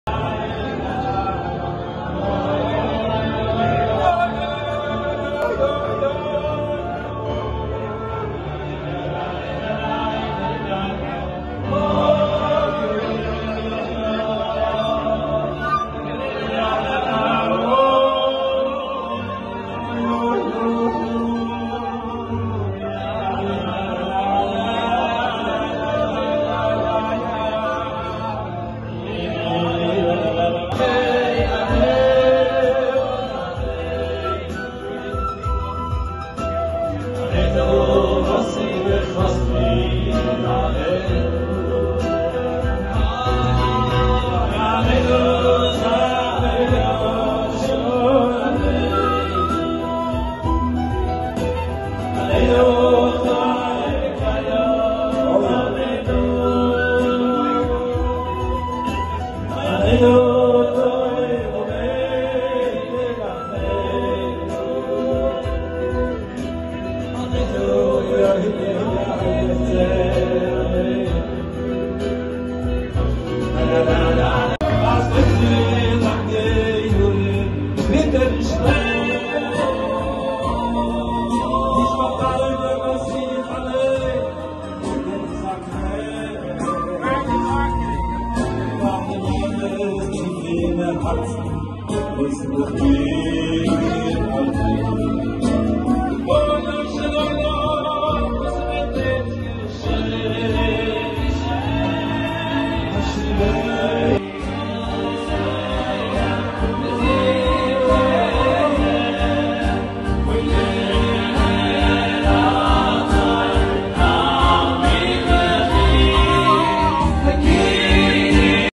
MBD • Shira Choir • Naftali Schnitzler | מבד, מקהלת שירה ונפתלי שניצלר
Magnificent Cruise featuring MBD and The Shira Choir led by Eli Shprei.